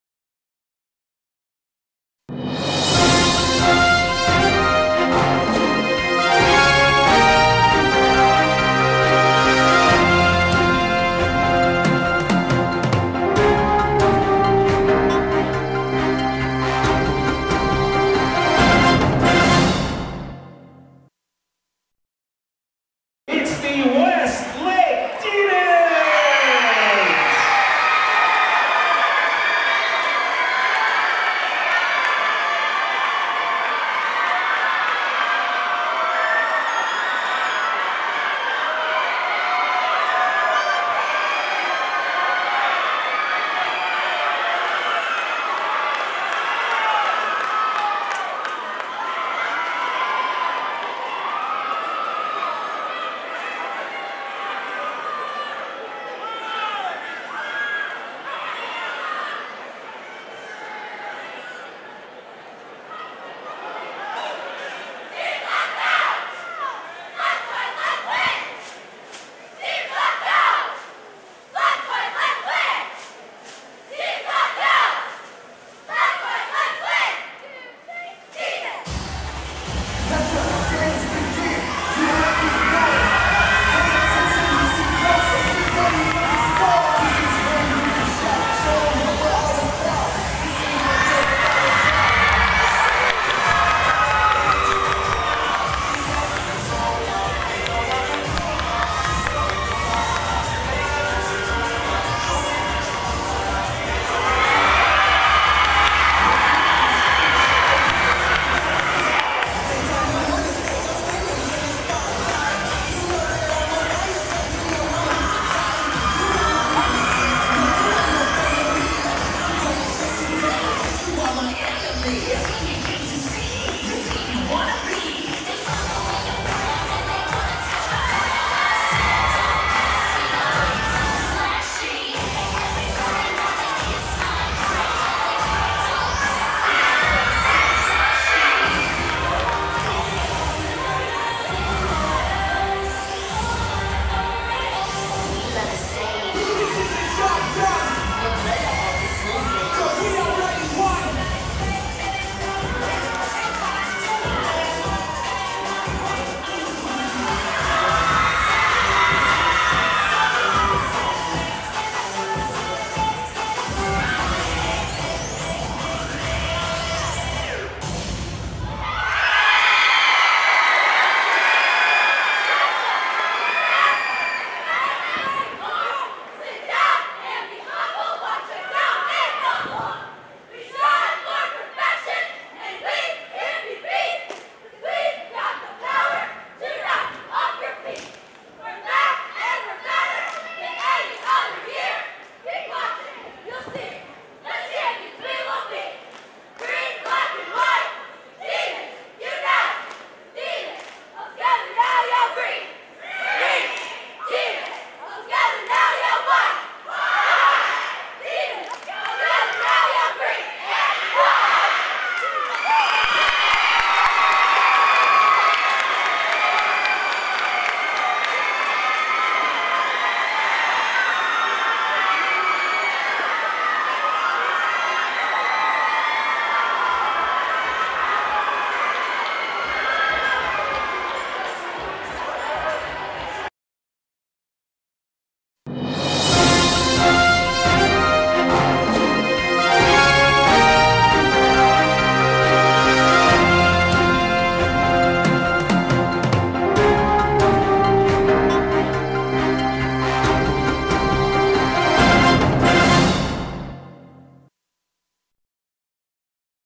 Westlake High School Cheerleaders SWC
Watch Westlake High School's Cheerleaders complete at their SWC competition!